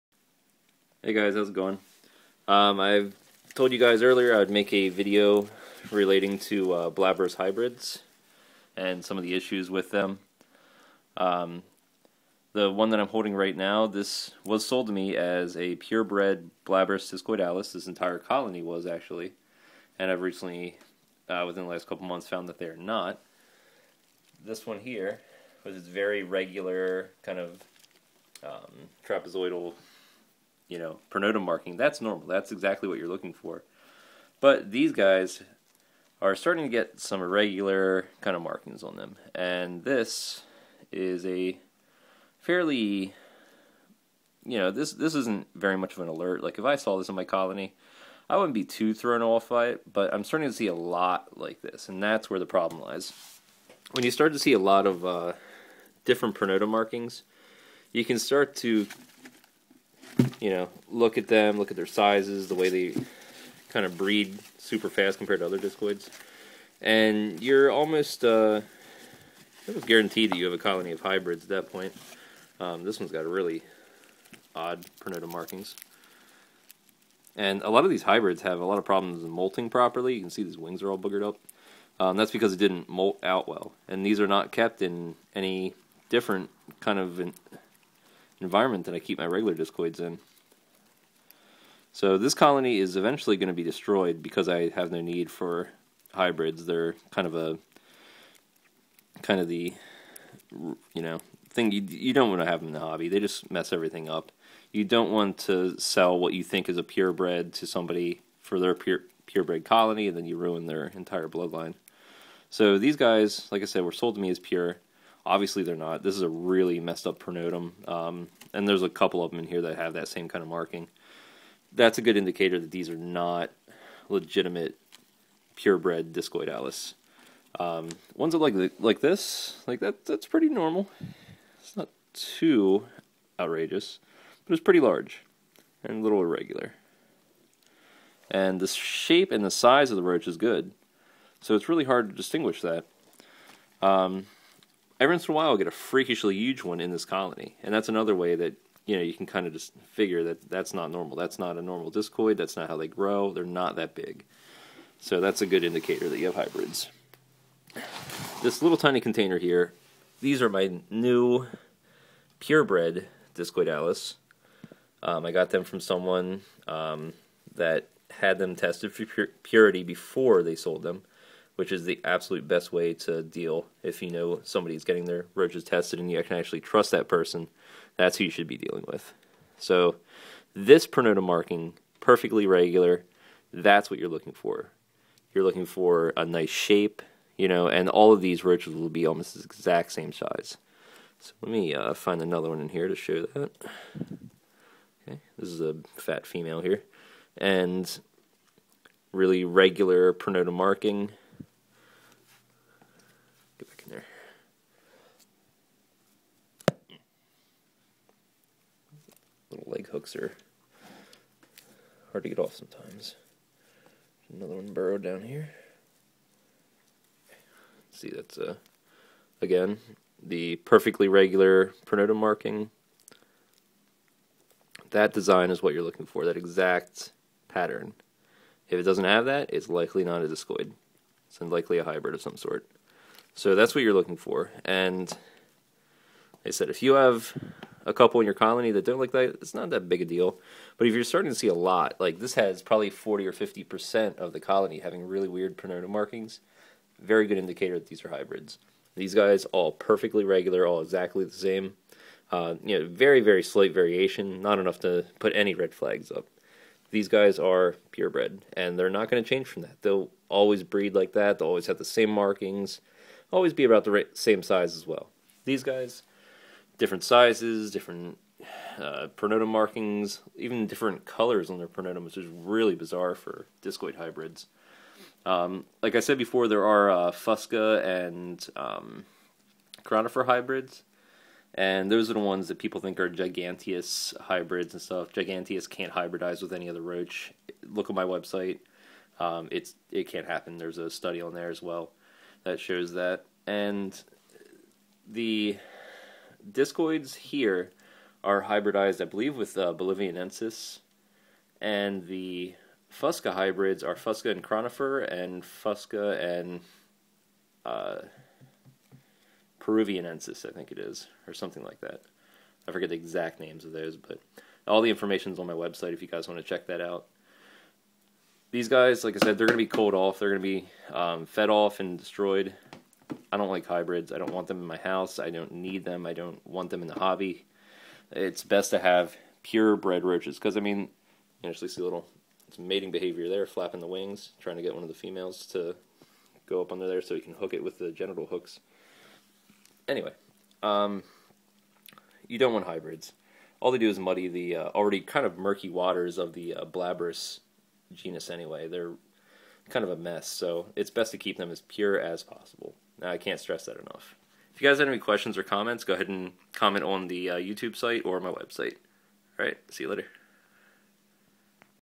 Blaberus hybrid comparison
This video is a comparison between hybrid Blaberus discoidalis and my new colony of pure bred Blaberus discoidalis.